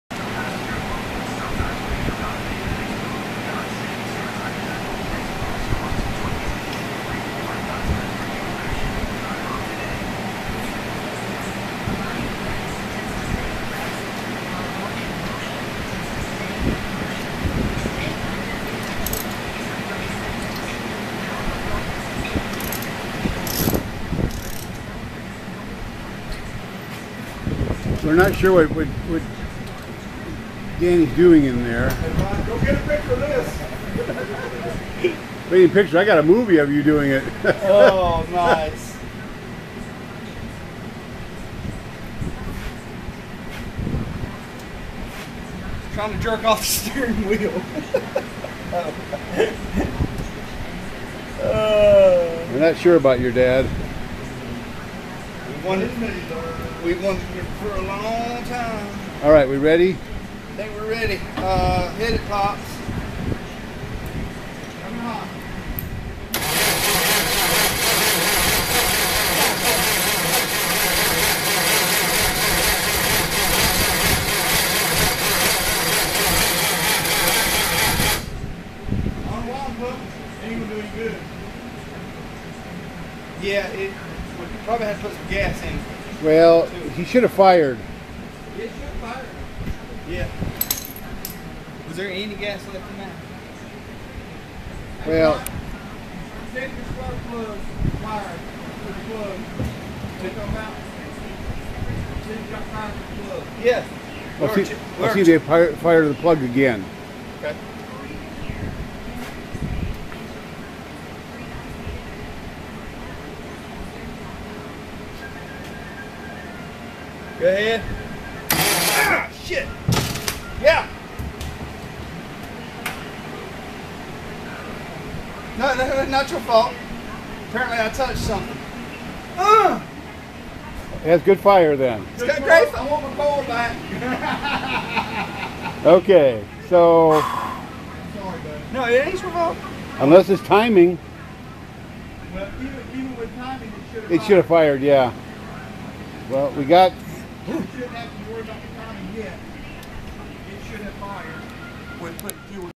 Crosley Pickup Truck
47 Crosley engine start - first attempt after finding/fixing ignition points dead short.  Good strong fire now - right Bryant?